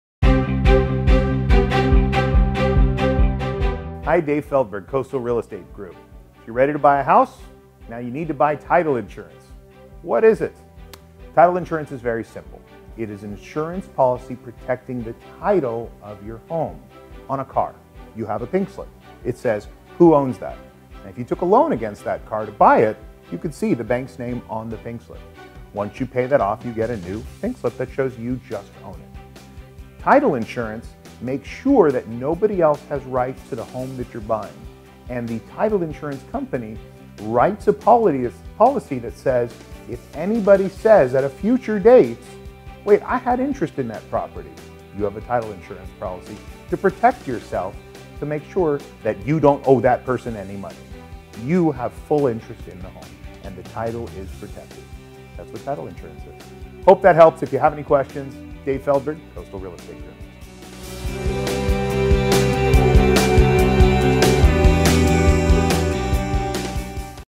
What is Title Insurance?
Title Insurance is one of the most important pieces of the real estate transaction yet most people have no idea what it is.  Listen to what Coastal Real Estate Group's broker/owner David Feldberg has to say on the topic.

For more information on buying or selling a home in Orange County, CA contact David Feldberg and search homes today!